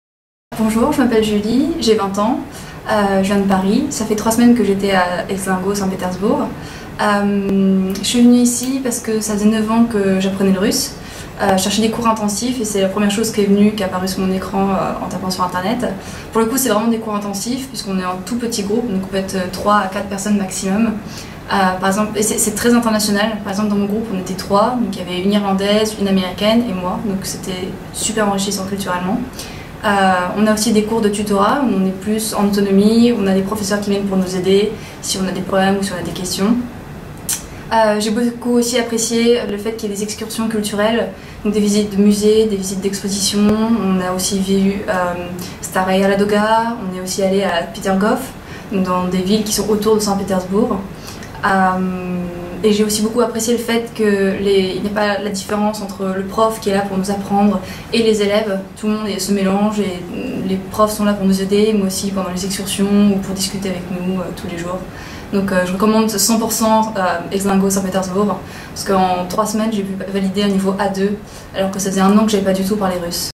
Julie sharing (in French) her Russian immersion experience at Exlinguo St Petersburg (summer 2017)
Julie is from France and has been studying Russian for 9 years. Having not practiced Russian for one year, she was looking for an intensive immersion course to regain the level she previously had. It was also the first time she was travelling to Russia. Благодарим Вас, Жюли, что поделились своим опытом о стажировке ExlinguoSPB этим летом! Надеемся, что Вы приедете еще в Россию!

Learn Russian in St Petersburg with ExlinguoSPB !